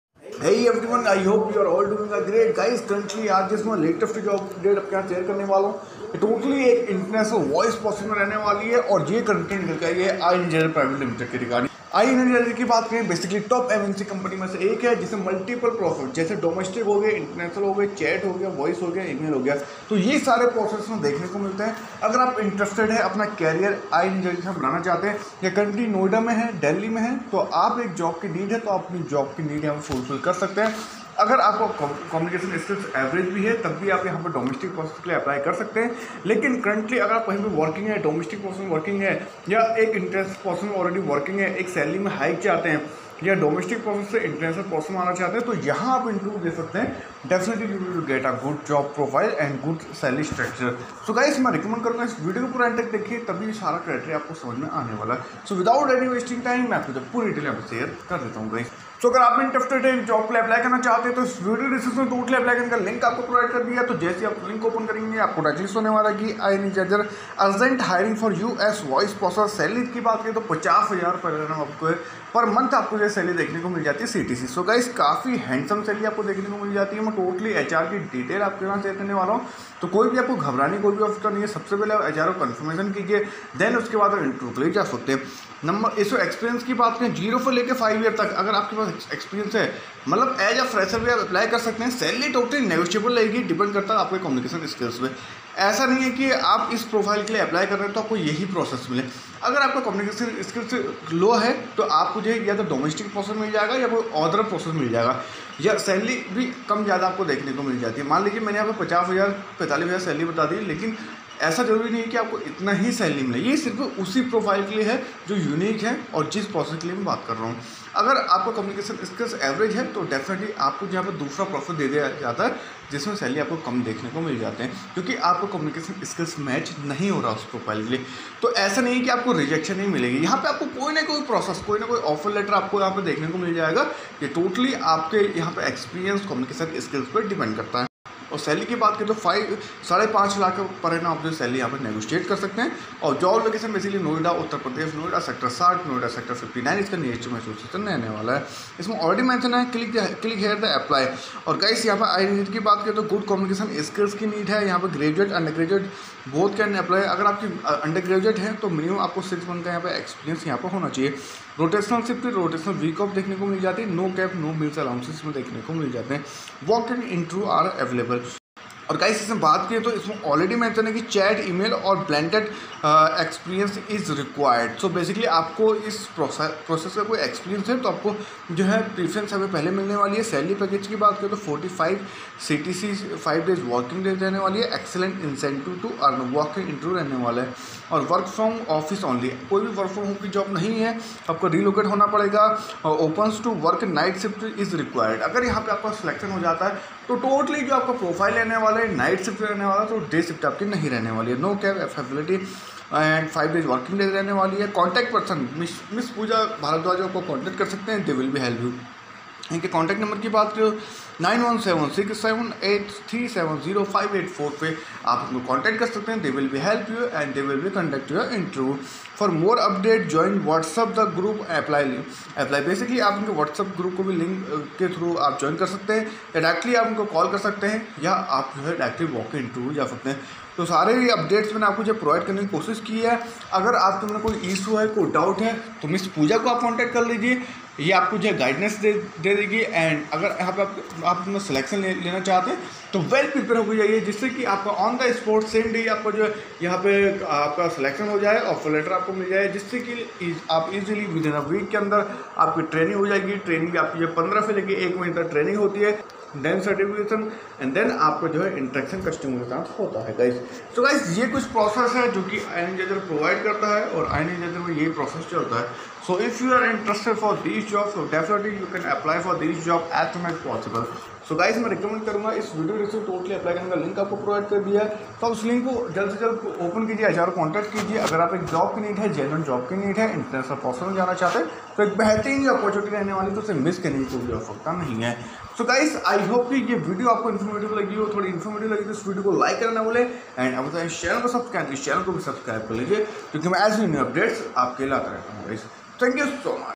Ienergizer Hiring International Voice Process || Best Process Jobs || MNC company in Noida / Delhi
Ienergizer Hiring for Blended voice process condidate interested condidate definitely apply for these job that is a very good opportunity to all of you guys so click the apply Link

Interested condidate apply for job as soon as possible

Thanks ❤️❤️